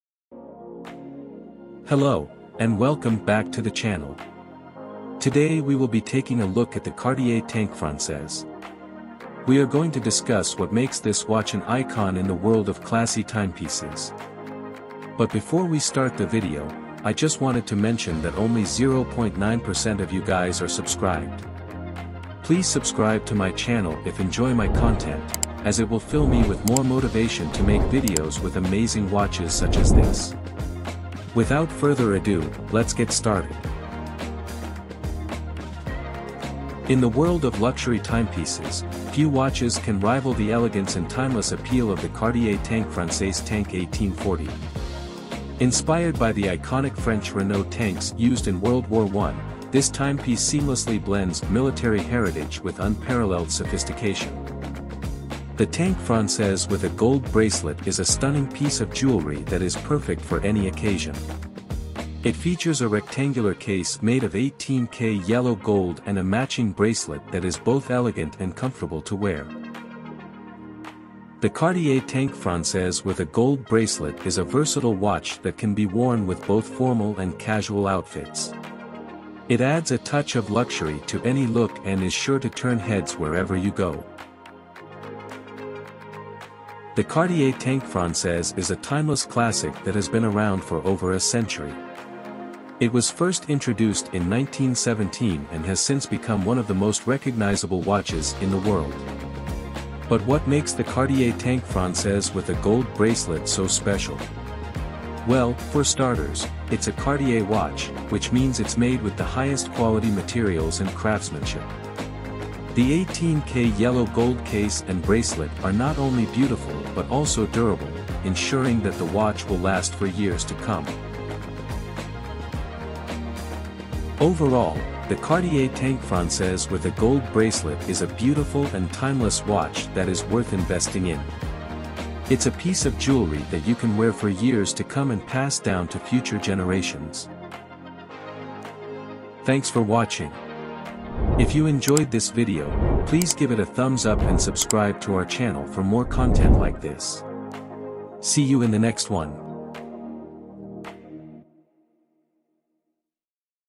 Why the Cartier Tank Francaise is a Must-Have for Luxury Watch Collectors.
The Cartier Tank Francaise is the epitome of luxury and sophistication. This timepiece features a sleek and elegant design, with a rectangular case made of 18K yellow gold and a matching bracelet that exudes opulence. The watch is powered by a self-winding automatic movement, ensuring accurate timekeeping without the need for manual winding. The white dial is adorned with Roman numeral hour markers and blue sword-shaped hands, adding to the watch's classic and timeless appeal. With its combination of exquisite craftsmanship and high-end materials, the Cartier Tank Francaise is a true statement piece that will elevate any outfit.